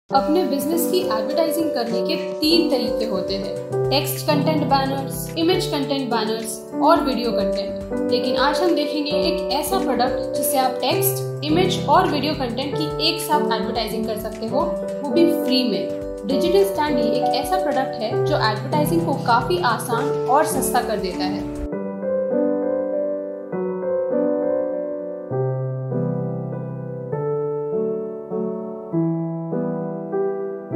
"Digital Standee: Elevating Engagement with Interactive Visuals"
digitalmarketing standee bestsignature delhi india indianteam promotion branding digital signagedesign youtube india design delhi, Digital signage is crucial for the future due to its ability to engage viewers, offer flexibility in content delivery, provide personalization, streamline operations, enable interactivity, allow for real-time updates and targeting, gather valuable analytics, and enhance branding and differentiation.